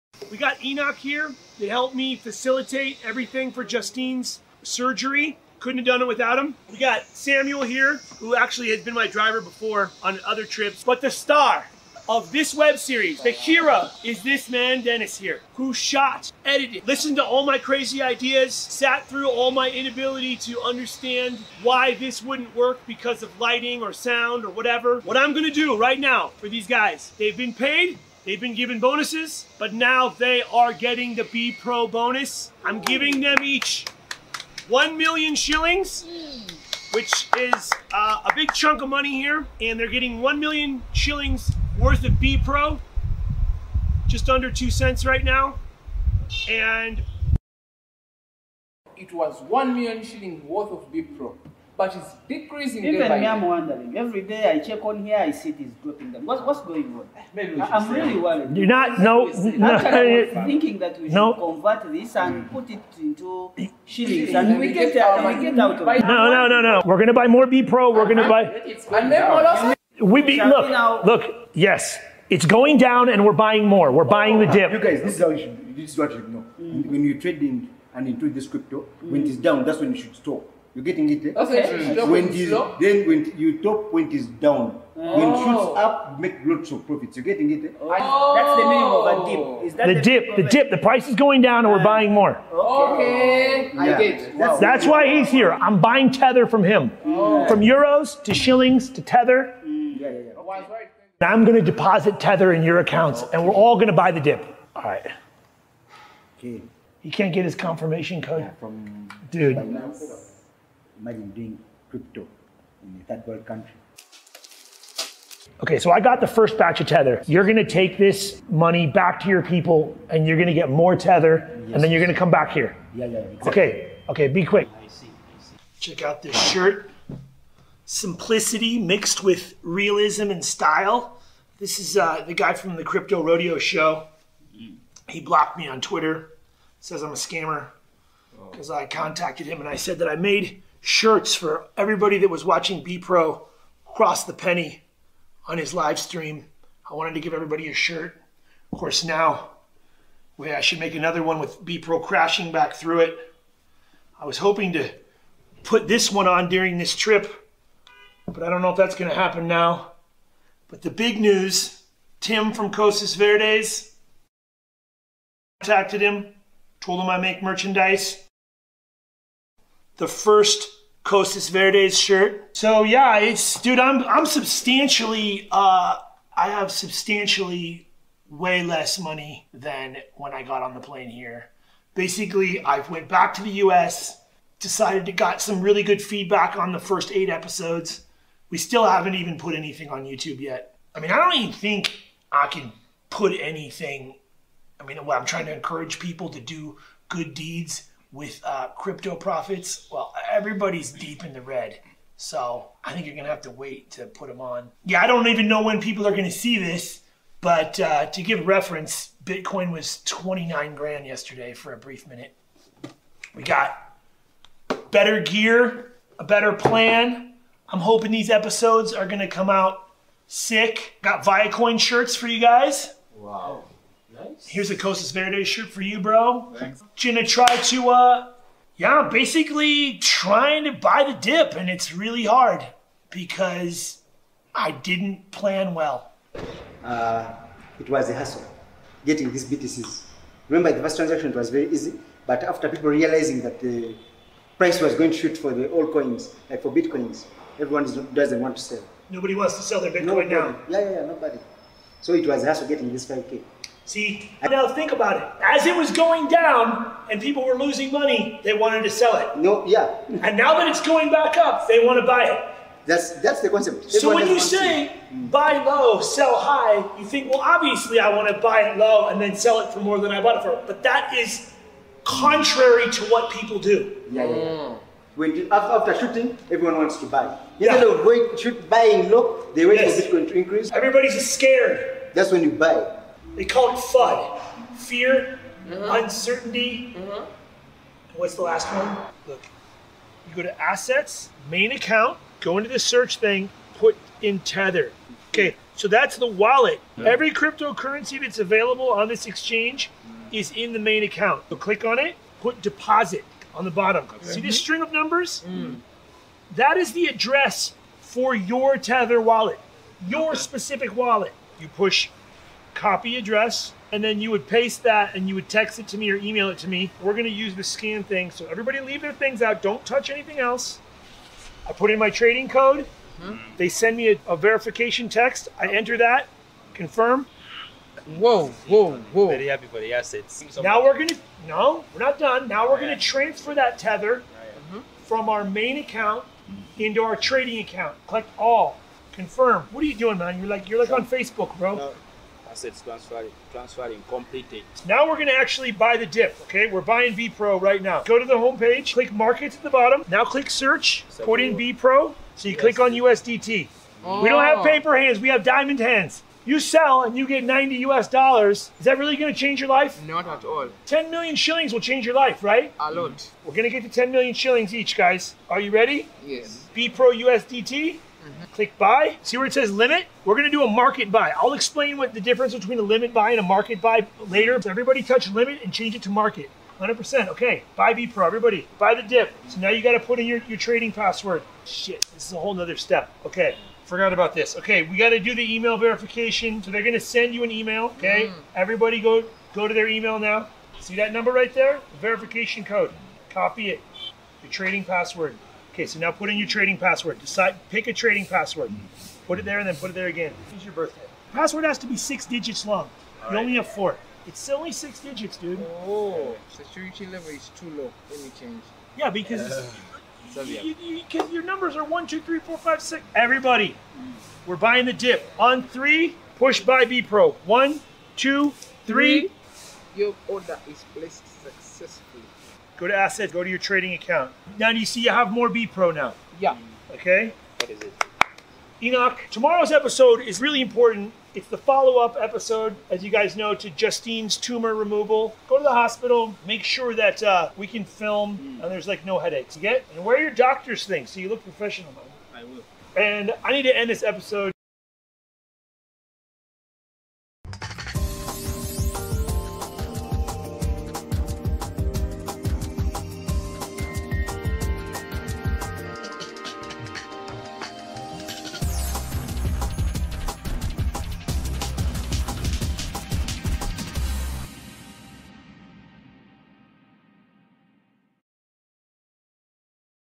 Buying the Bepro Dip The Altruistic Crypto Adventure Show S2 EP1
We got Enoch here. They helped me facilitate everything for Justine's surgery. Couldn't have done it without him. We got Samuel here who actually had been my driver before on other trips, but the star of this web series, fakira is this man Dennis here who shot, edited, listen to all my crazy ideas sat through all my inability to.

Understand why this wouldn't work because of lighting or sound or whatever. What I'm going to do right now for these guys. They've been paid, they've been given bonuses, but now they are getting the bepro bonus I'm giving them each.

1,000,000 shillings, which is a big chunk of money here, and they're getting 1,000,000 shillings worth of bepro. Just under $0.02 right now and.It was 1,000,000 chilling worth of Big Bro, but it's decreasing. Even now I'm wondering every day I check on here. I see this. Maybe not no, no help.

I'm really worried.

After thinking that we convert this and put it to. No no no no. Right? We're gonna buy more people.

We beat look, look. Yes it's going down and we're buying more. We're buying the dip.

Oh, you guys know when you trade in and into this crypto, which is down, that's when you should store your getting it when you then when you talk point is done.

When shoots up make good profits. Like that's the meaning of a deal is that. Dip the dip.

The price is going. Down and we're buying more. OK, I guess wow. Yeah, that's that's why he's here.

I'm buying tether from him from euros to schillings to tether, yeah.I'm going to deposit tether in your accounts and we're all going to.Buy the dip right?

Dude, you can't get his confirmation code from dude. Right now. Magdin crypto in the third world country.OK so I got the first batch each other. You're going to take this money back to your people and you're going to get. More tether and Then you're going to come.

Back here. OK OK be quick .Check out this shirt.

Simplicity mixed with realism and style, this is the guy from the crypto radio show.

He blocked me on Twitter says I'm a scammer because I contacted him and I said.

And I said that I made shirts for everybody that was watching bepro across the penny on his live stream.

I wanted to give everybody a shirt of course now. Wait, I should make another one with Bipro crashing back through it? I was hoping to put this one on during this trip, but I don't know if that's going to happen now. But the big news Tim from Kocis Verdes attacted him told him I make merchandise.

The first kocis marinade. So yeah, it's dude.

I'm I'm substantially, uh, I have substantially way less money than when I got on the plane.

Here basically I've went back to the US, decided to got some really good feedback on the 1st 8 episodes.

We still haven't even put anything on YouTube yet I mean, I don't even think I can put anything.

I mean well. I 'm trying to encourage people to do good deeds with crypto profits.

Well, everybody is deep in the red, so I think you're gonna have to wait to put him on.Yeah, I don't even know when people are going to see this, but to give reference Bitcoin was 29 grand yesterday. For a brief minute we got better gear, a better plan.

I'm hoping these episodes are going to come out sick got viacoin shirts for you. Guys, wow here's the closest Verde shirt for you bro Jenna tried to a yeah basically trying to buy the dip and it's really hard because I didn't Plan well.

It was a hassle getting these businesses remember the last transaction was very easy, but after people realizing that the price was going to